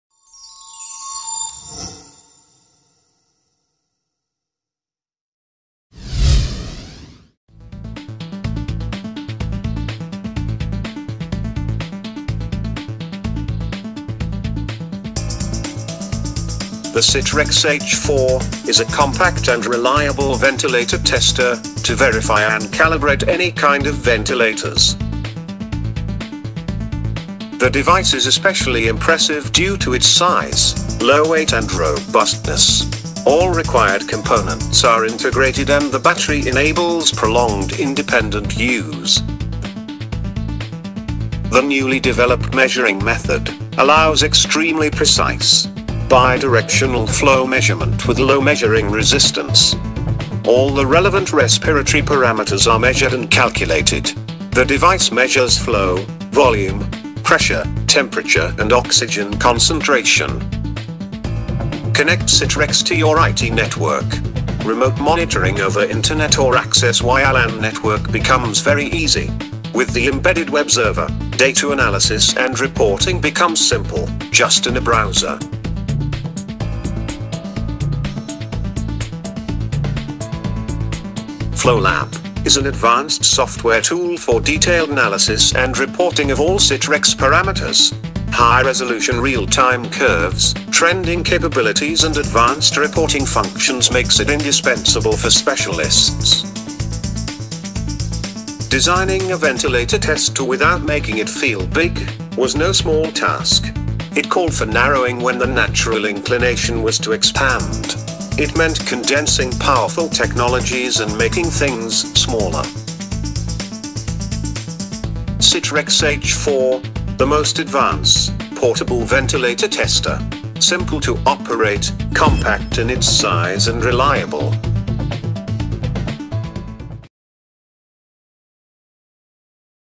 CITREX H4 portable Ventilator Tester
The most precise gas flow analyzer and pressure test instrument for mobile use. The test tool of choice for healthcare quality and safety professionals.
CITREX H4 is designed for easy mobile use while still meeting all the requirements of day-to-day field operations. Its capabilities allows it to analyze the performance of a wide variety of medical devices. Examples of gas flow or pressure producing devices are respiratory ventilators, endoscopic insufflators, anesthetic machines, oxygen flow meters, pressure gauges and suction devices.